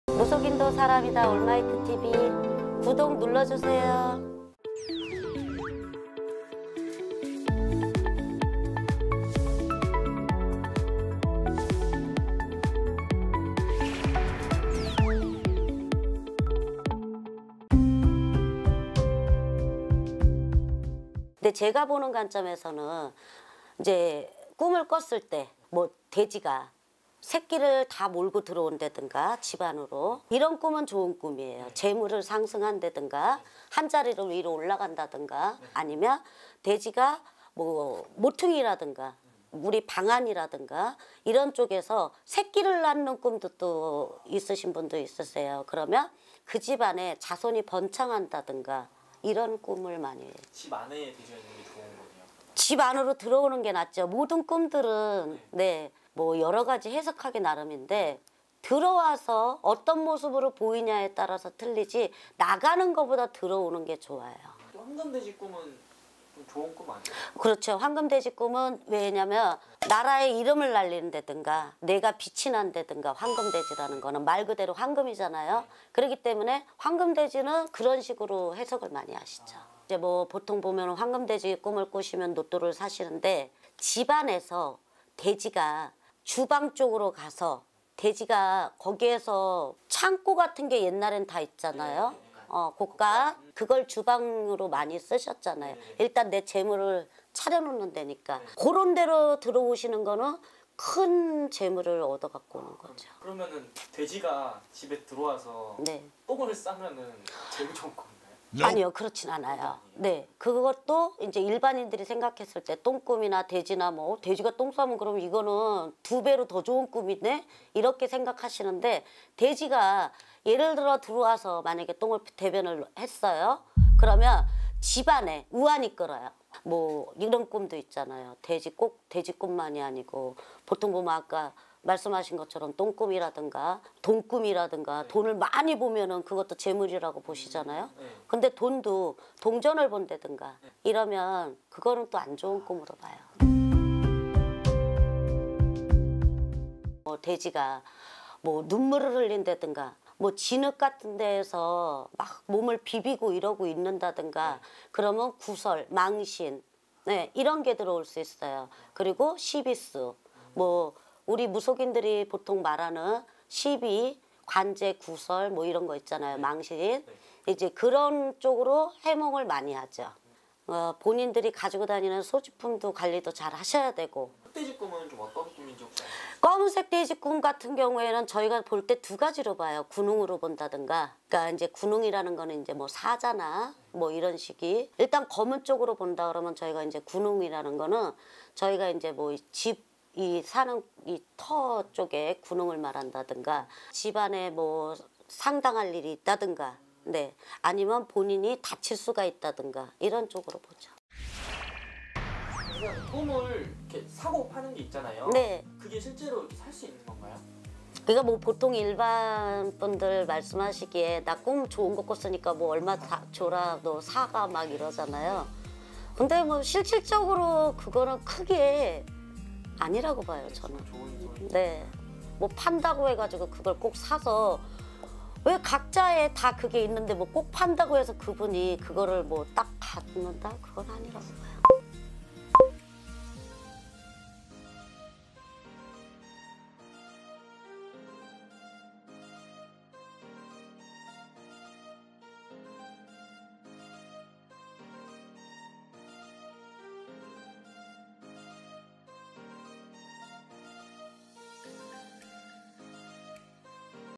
돼지꿈은 최고의 꿈? 무조건 로또를 사야된다?? 돼지꿈 해석 총정리 하남접집 송파점집
자막 : 나눔템플릿 ★하남점집★
- 위치 : 경기도 하남시 덕풍동 356-4

★대표블로그★
-올마이티TV